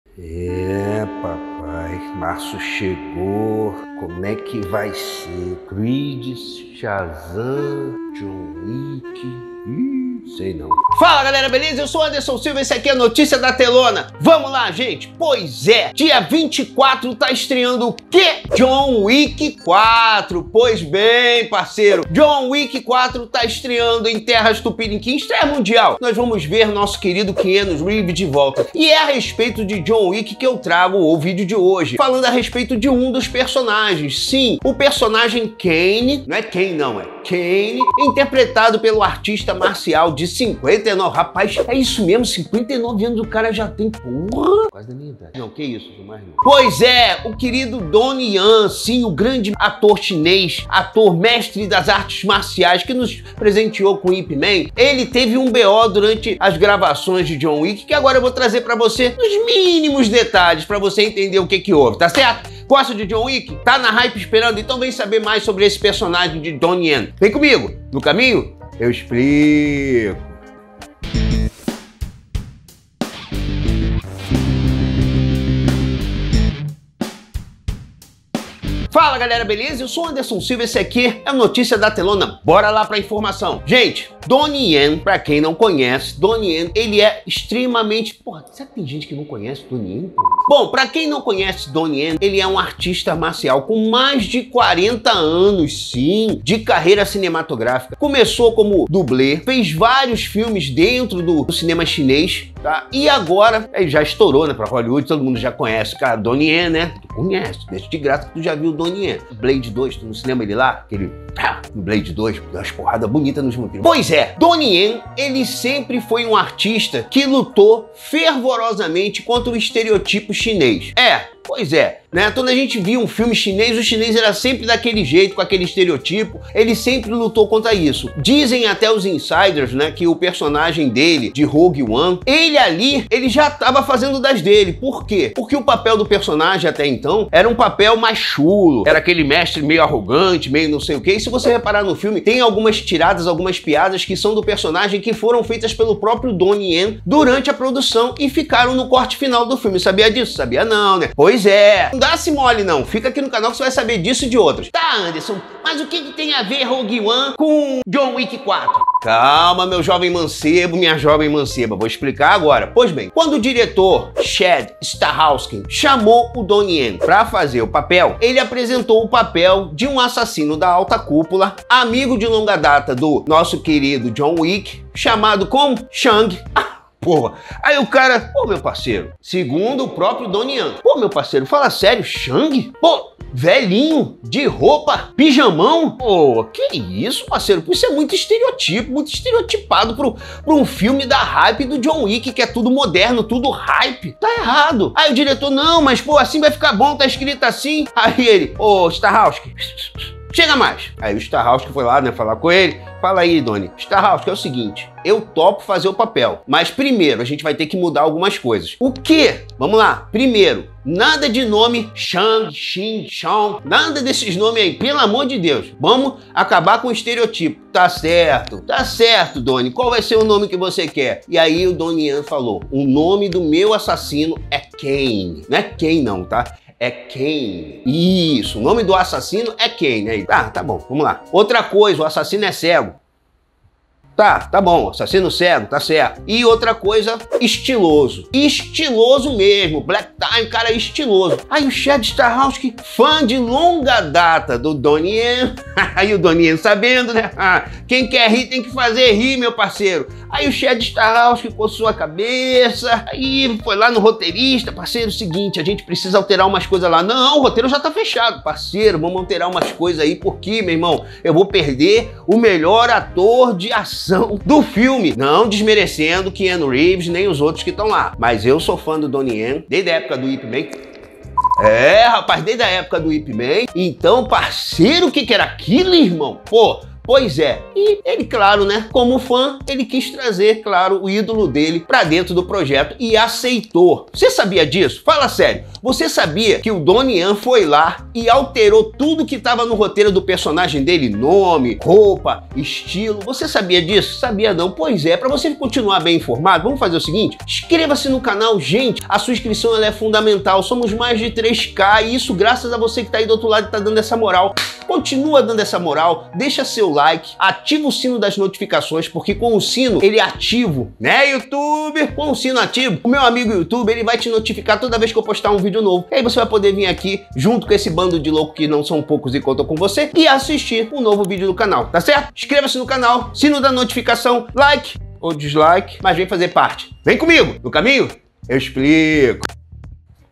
JOHN WICK 4 - ENTENDA PORQUE DONNIE YEN DISSE "NÃO!"
A estrela de John Wick: Capítulo 4, Donnie Yen, revela os ajustes que fez em seu personagem Caine no próximo filme de ação para erradicar o estereotipo asiático de Hollywood.
Vem comigo que no caminho te explico.